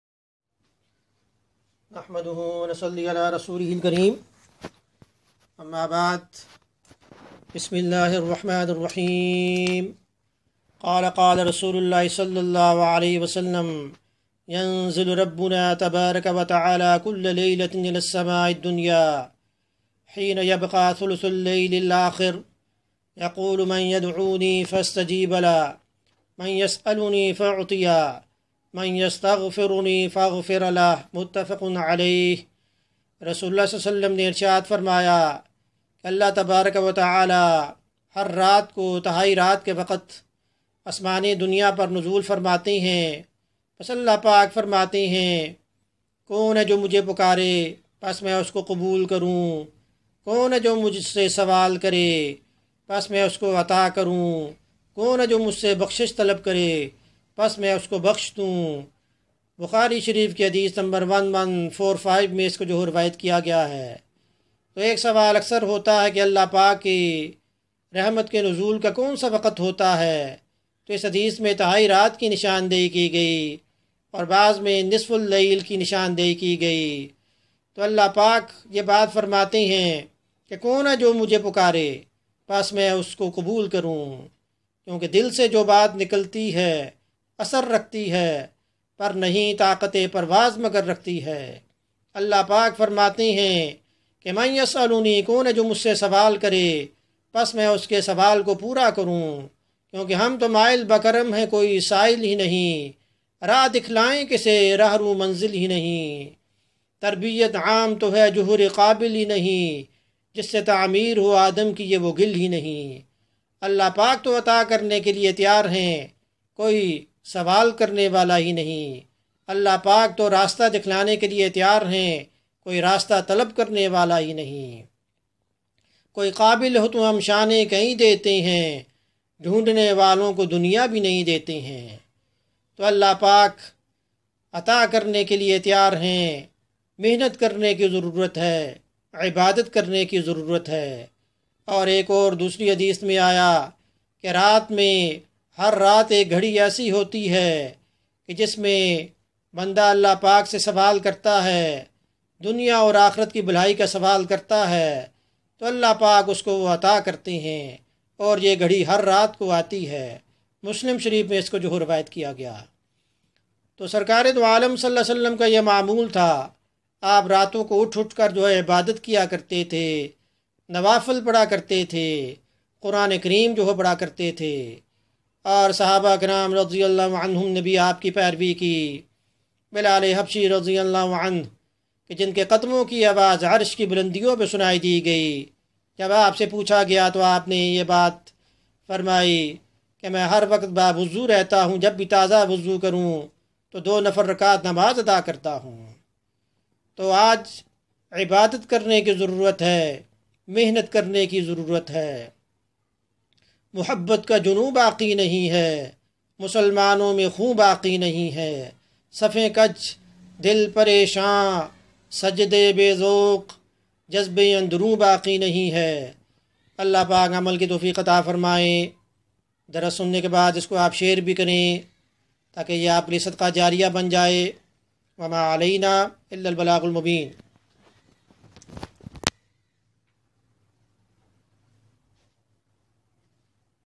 darse hadith || Allah ki Rehmat kab nazil hoti ha
in the third of night there come a time for acceptance of Dua.